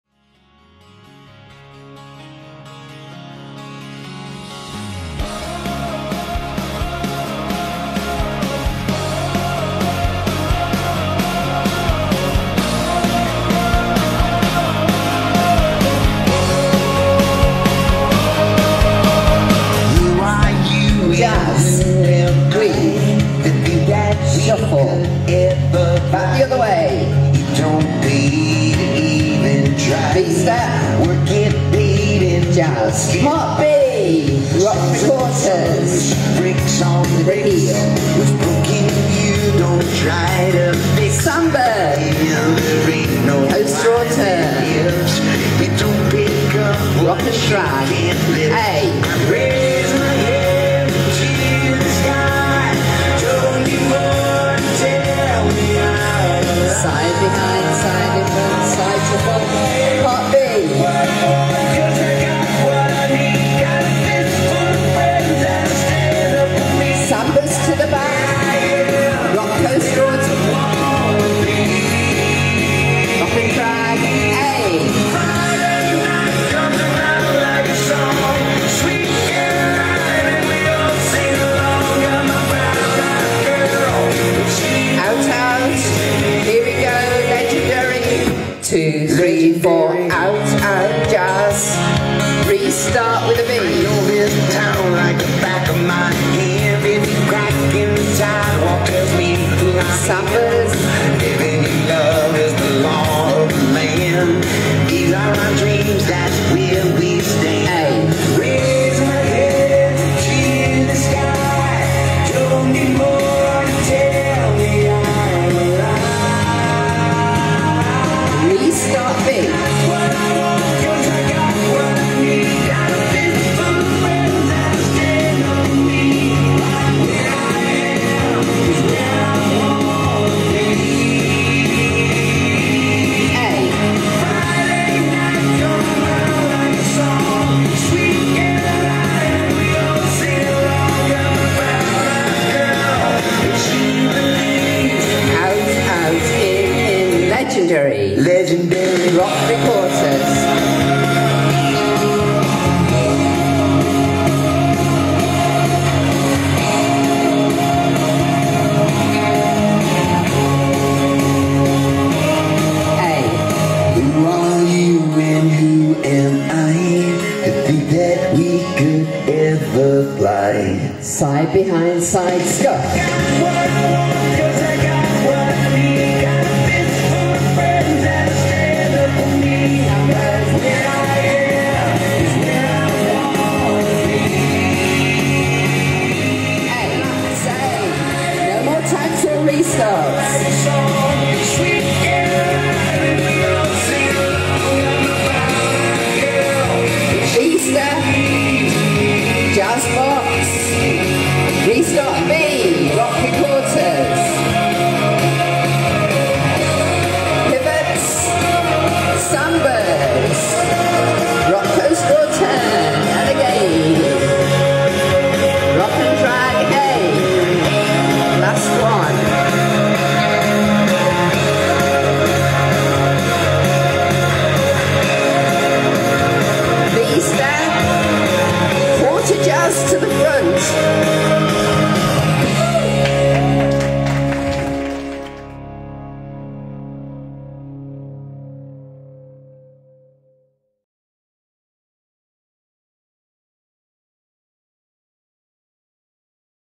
LEGENDARY - Workshop Demo
LEGENDARY - Ready Set Dance 2024 Workshop Demo
Choreographed by Alan Birchall & Jacqui Jax
Phrased 64 Count  4 Wall Intermediate
Released at 'Ready Set Dance Benidorm 2024'